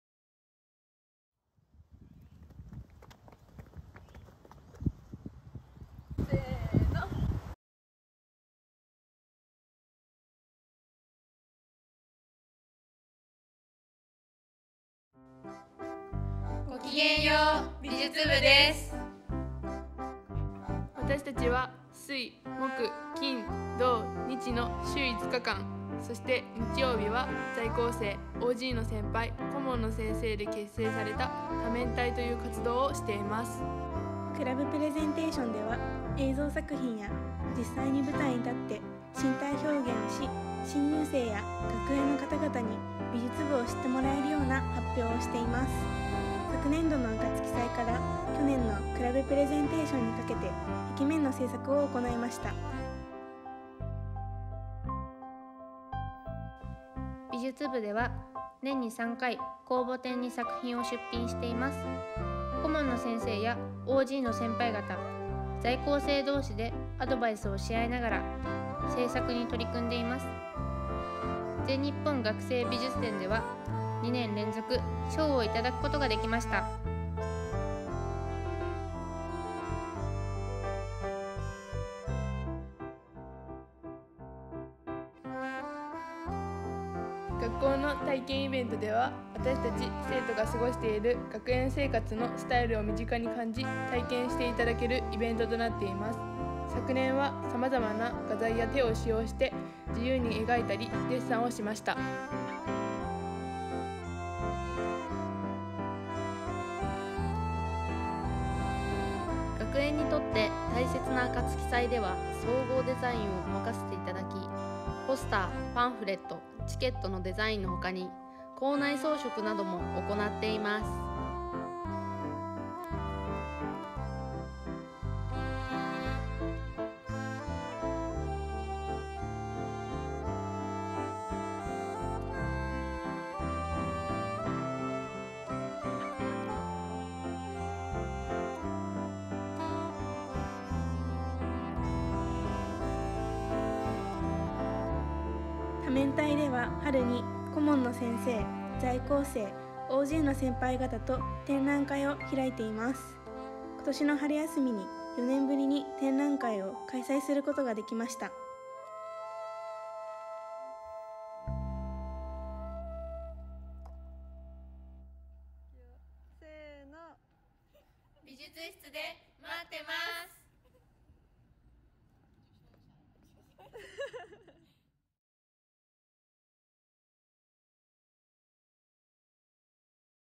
2023 美術部 クラブプレゼンテーション
ごきげんよう、瀧野川女子学園高等学校 美術部です。

新入生に向けたクラブプレゼンテーションにて紹介した動画をお届けします！
今回の発表では、「美術部を発見しよう！」というキャッチフレーズで一年間の主な活動の様子を楽しい音楽と一緒に動画へ詰め込みました。

〈クラブプレゼンテーションについて制作したブログ〉
クラブプレゼンテーションを行いました！

2023年クラブプレゼンテーションに向けて準備中！